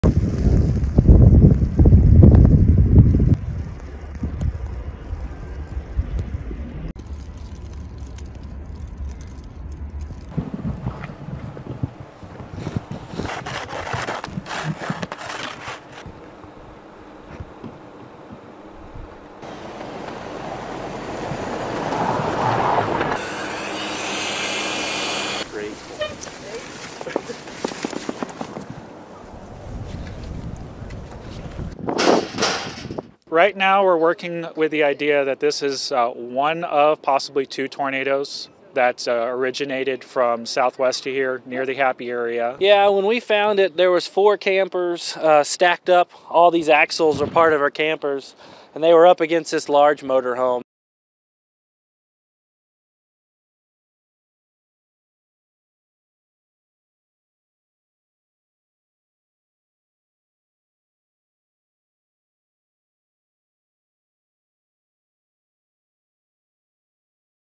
Deep snow in Rockies, Possible tornadoes in Texas
A major snowstorm is making travel hazardous in the central Rockies, grounding flights and closing roads. Possible tornadoes hit the Texas Panhandle over the weekend. (March 15)